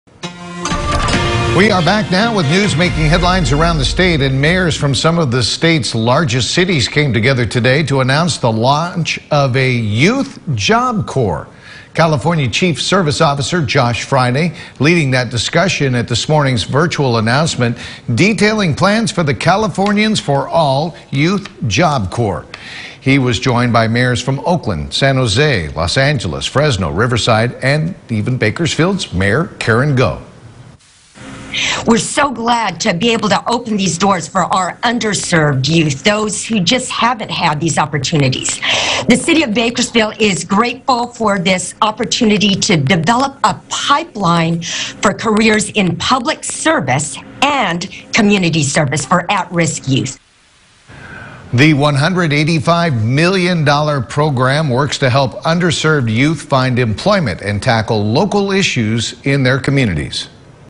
Mayors, including Bakersfield's Karen Goh, announce formation of Californians For All Youth Job Corp
Mayors, including Bakersfield's Karen Goh, announce formation of Californians For All Youth Job Corps